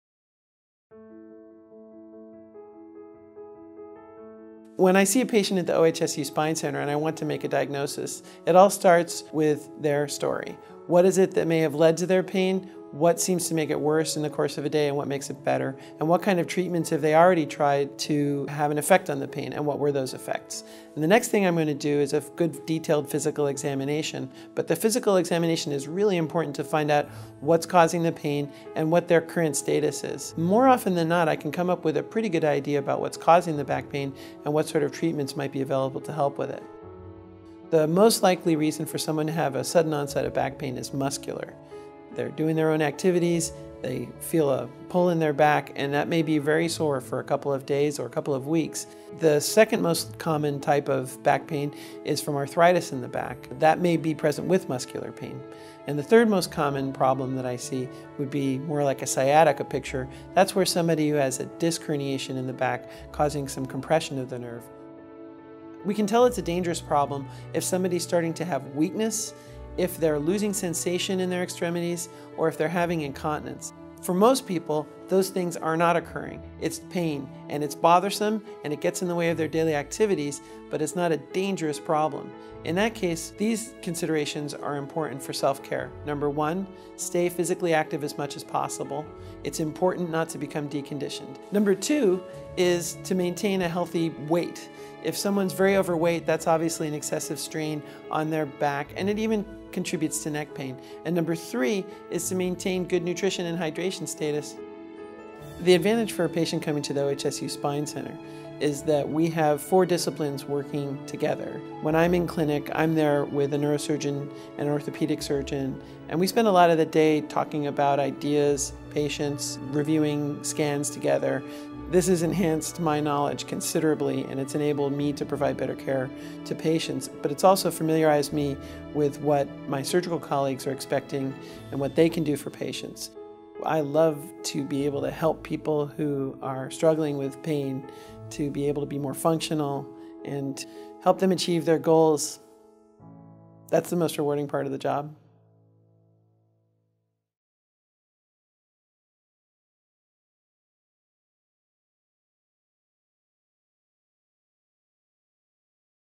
What causes back pain?
David M Sibell, M.D., with the OHSU Spine Center discusses causes and treatments for back pain.